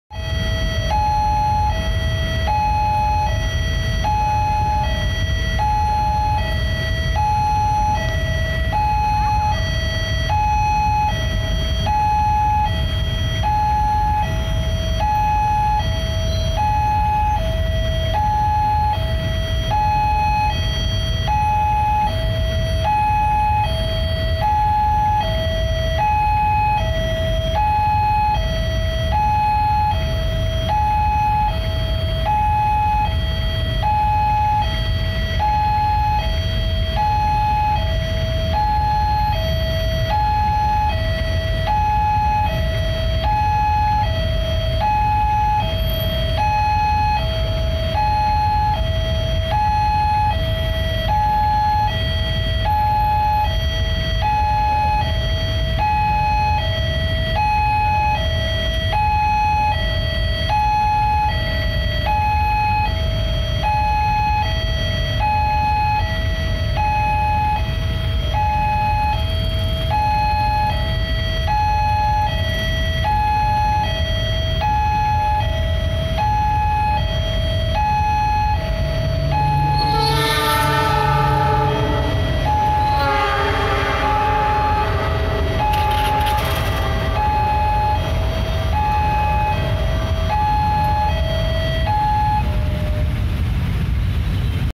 Stop!!! Ular Besi (Kereta Api) Sedang Lewat di PJL RSI Wonokromo || Indonesian Train Compilation
Pas lewat jalan ini tiba-tiba terhenti karena ada Kereta Api yang akan lewat. Rekam dulu ahhh.